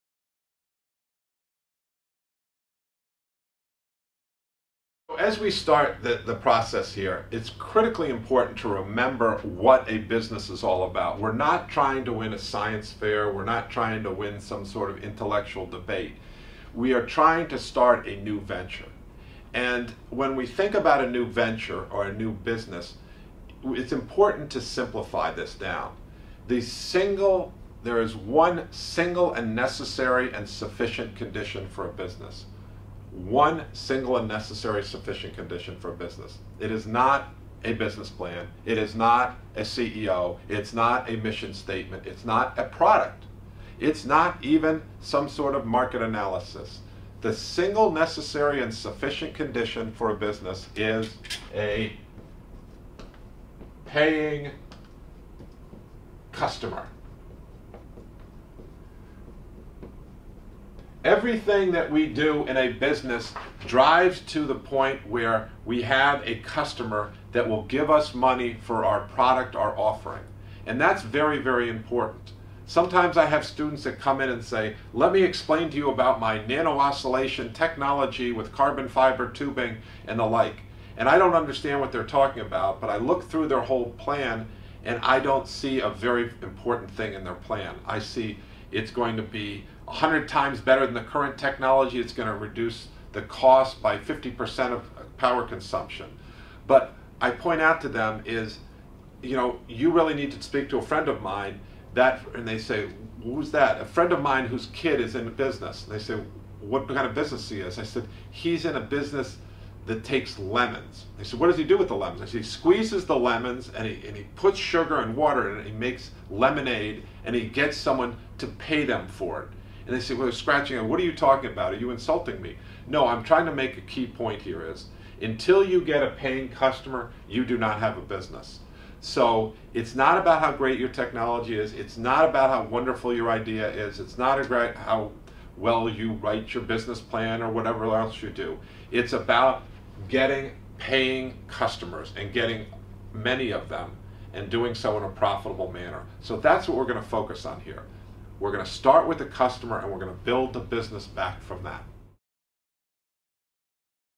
What Makes a Business
MIT 15.390 New Enterprises, Fall 2013
Instructor: Bill Aulet

Discussion of what is the single necessary and sufficient condition for a business and why it is so important for aspiring entrepreneurs to focus primarily on this condition.